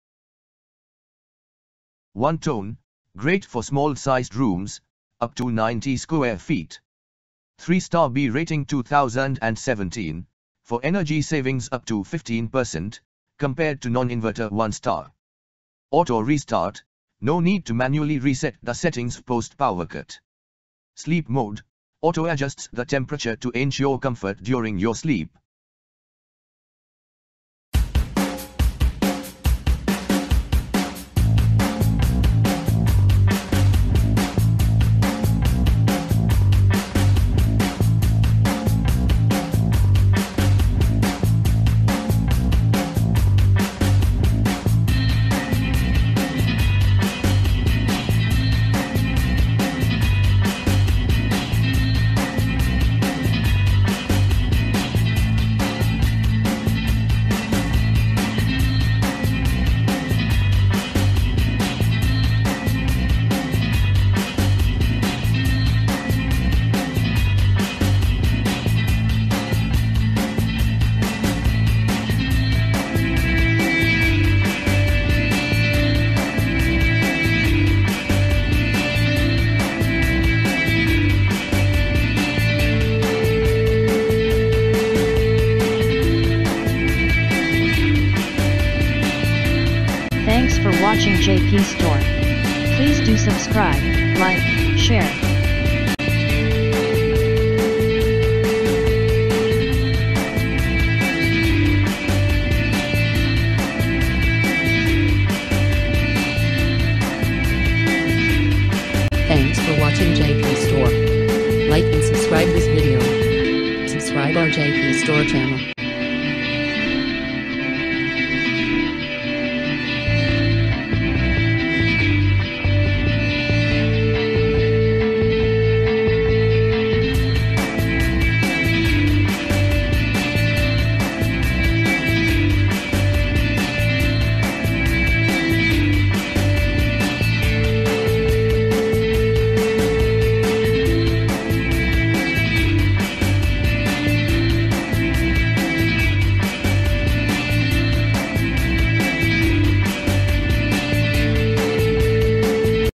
Voltas 1 Ton 3 Star Split AC - White(123 LYB)
1 Ton : Great for small sized rooms (upto 90 sq ft)
3 Star BEE Rating 2017 : For energy savings upto 15% (compared to Non-Inverter 1 Star)
Auto Restart: No need to manually reset the settings post power-cut
Sleep Mode: Auto-adjusts the temperature to ensure comfort during your sleep